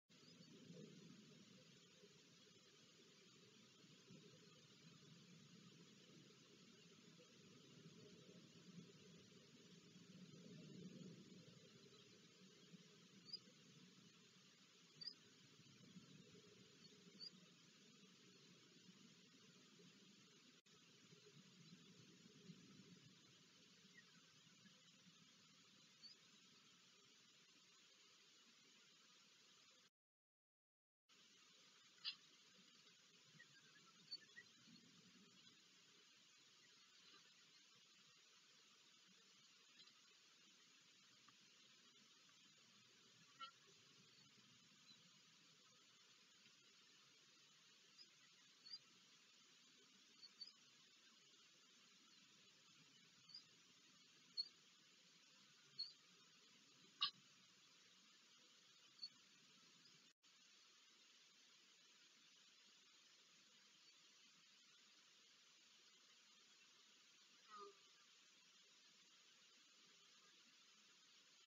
Kurhannik(Buteo rufinus) IZRAEL - zmiana, samiec odlatuje 2024 03 01
Projekt „Live Birds of Prey”  jest realizowany za pozwoleniem  Zarządu Przyrody i Parków,